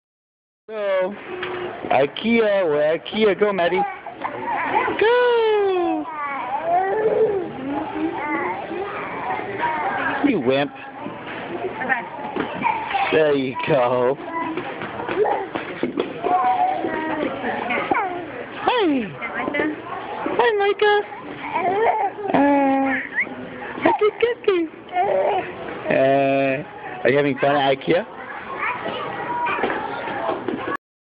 Maddie, Micah, Mommy and Daddy at IKEA
Girl has fun anywhere. loves the slides.